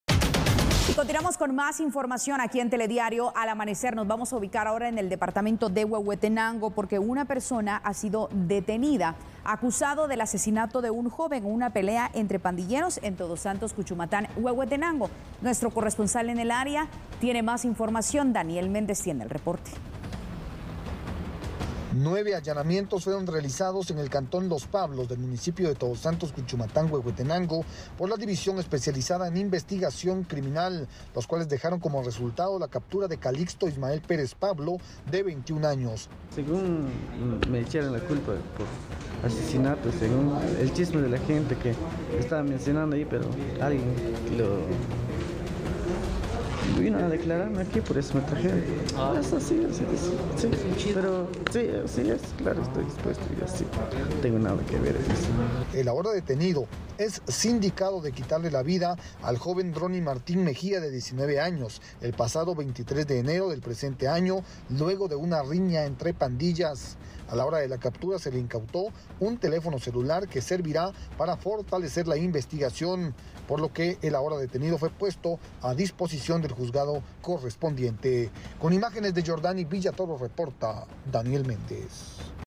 Presunto asesino de joven es capturado en Huehuetenango
Debido a unos allanamientos en Huehuetenango, la PNC logró capturar a un joven de 21 años, que es acusado de asesinar a otro dos años menor que él. Esto se habría dado durante una riña entre pandillas.